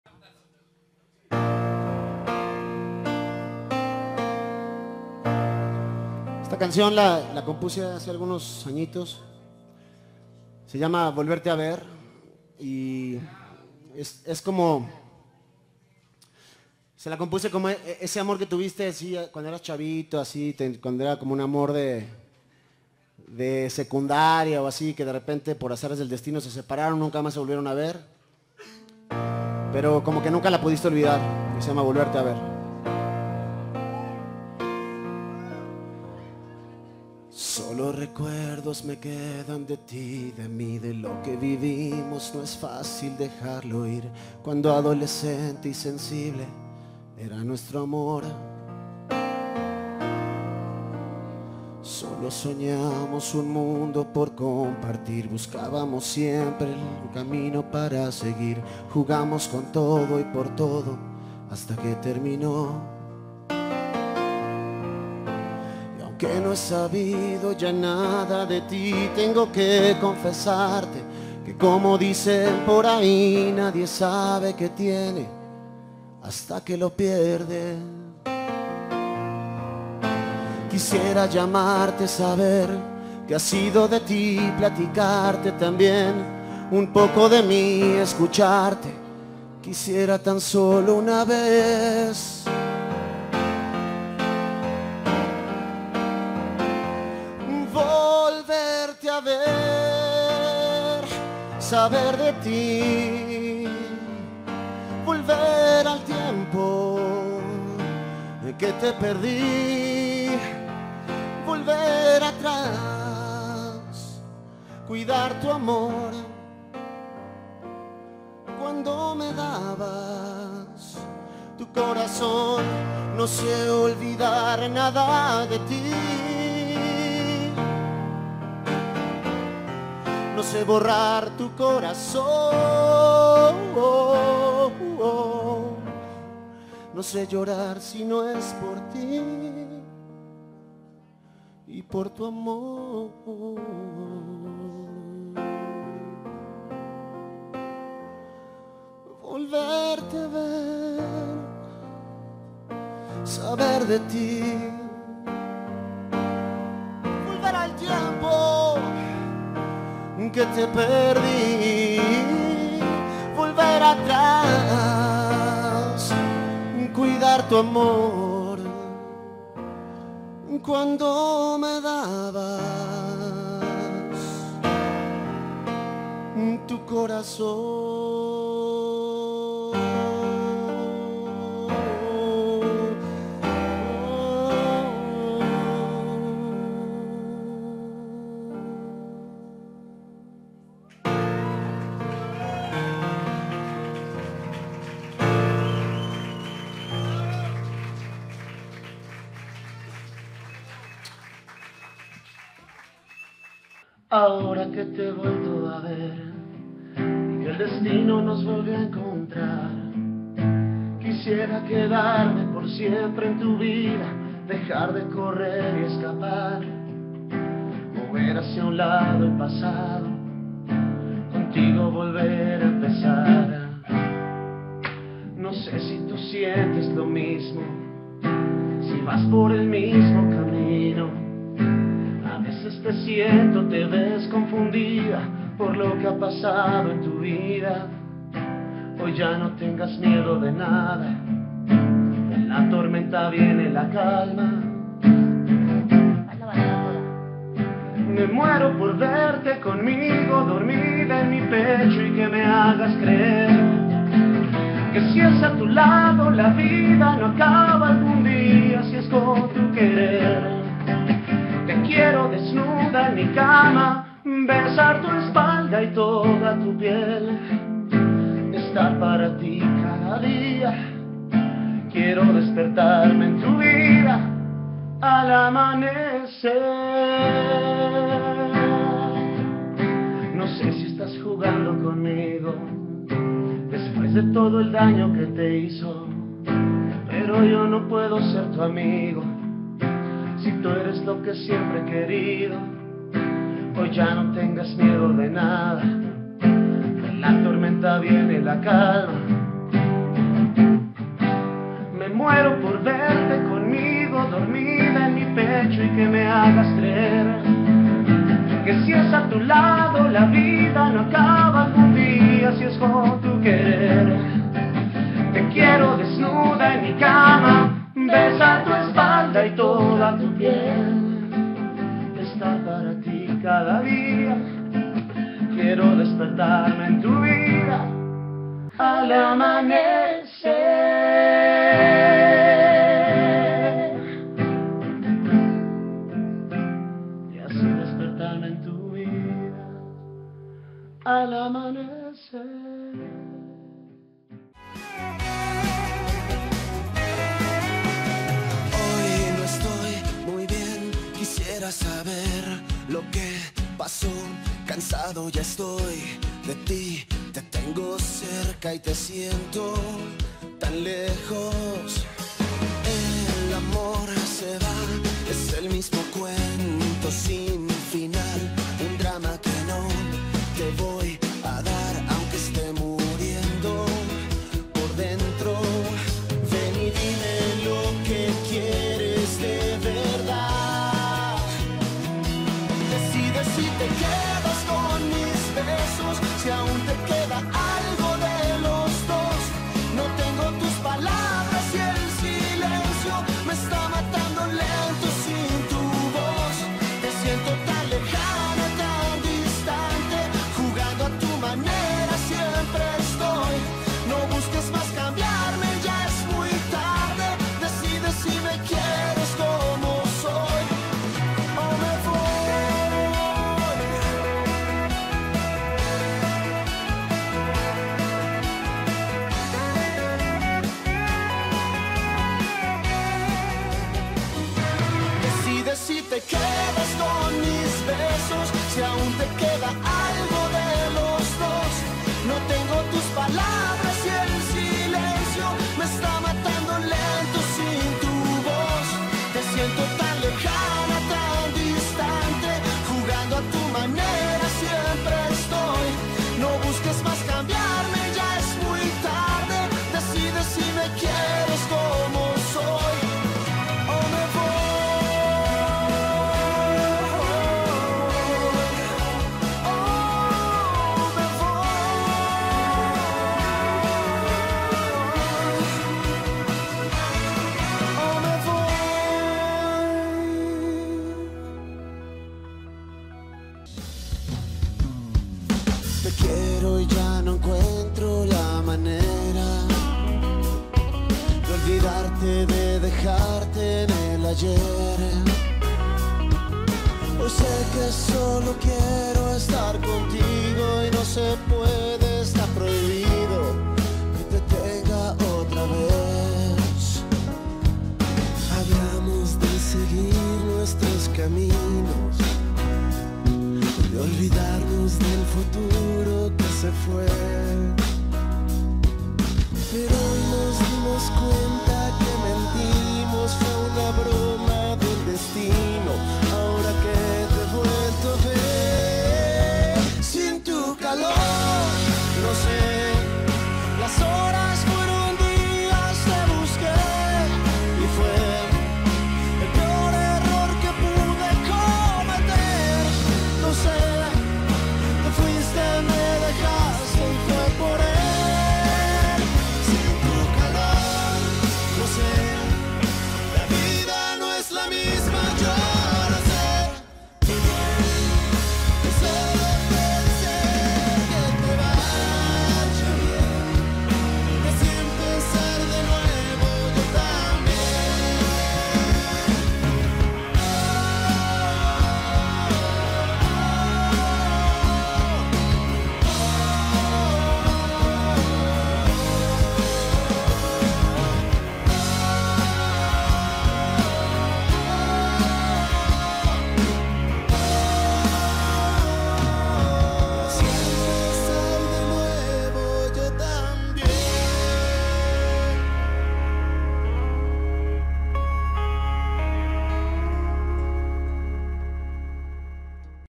Canciones de Federico Vega para Lidia Avila (OV7)
Federico Vega es un gran compositor mexicano apoyen su música. Aunque las separaciones son dolorosas hay que sacar lo mejor, y en este caso reconocer estas tremendas canciones, compren sus discos y busquen su música en YouTube y plataformas digitales.